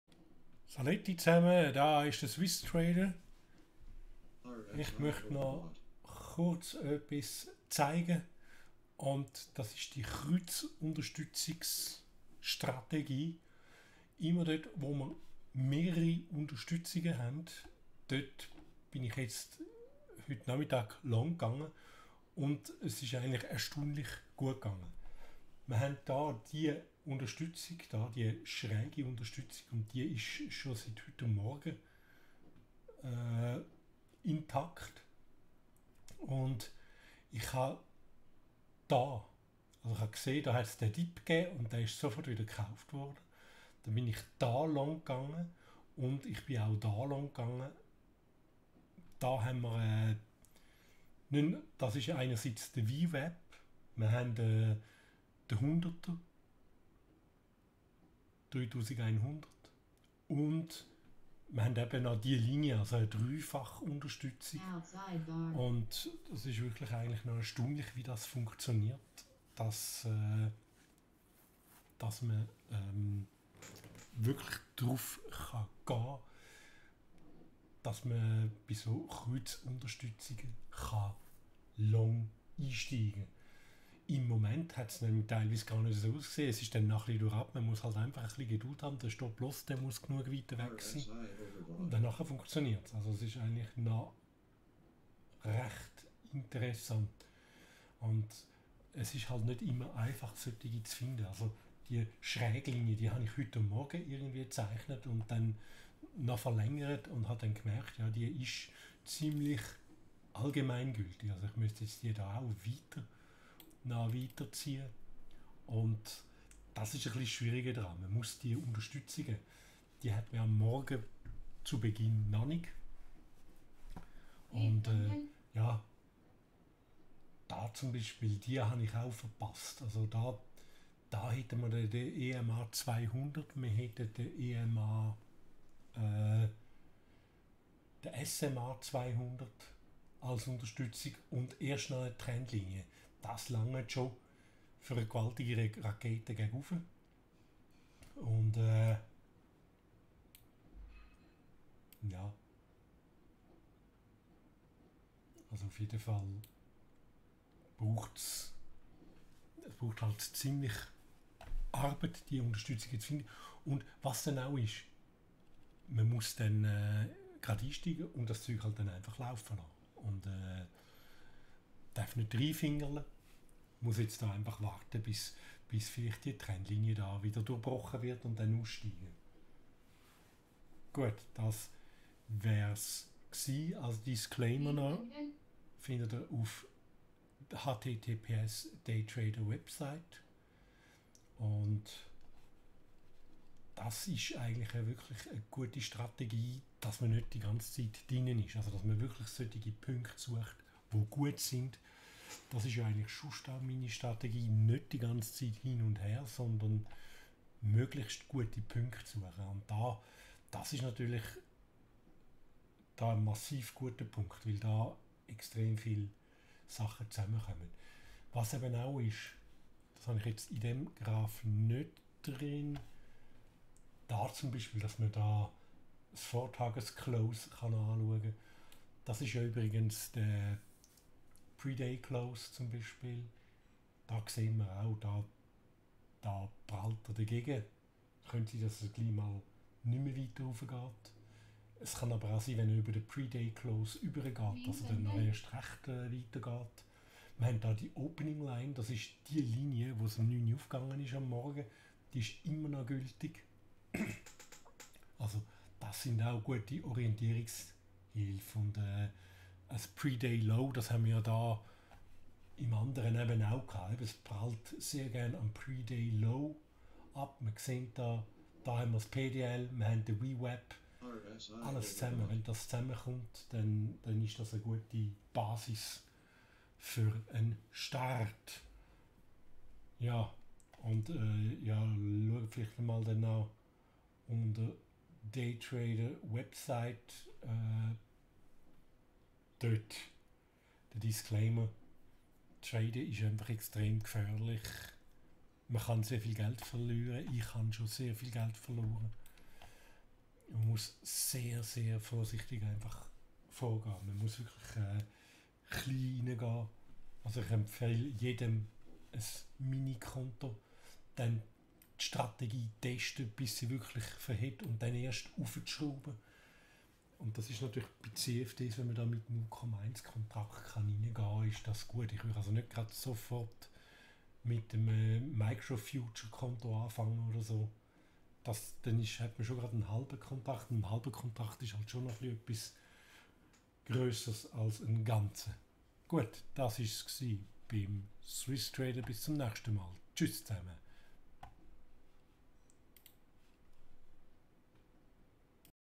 Die Kreuzunterstützungs-Strategie eignet sich für lahme long Märkte.
Vortagestief, VWAP und Trendlinie: Falls diese zusammenfallen spricht man von einer Kreuzunterstützung. Viele Trader und Roboter erkennen diese Konfluenz und starten einen Trade. Die Wahrscheinlichkeit eines Erfolgs ist sehr gross.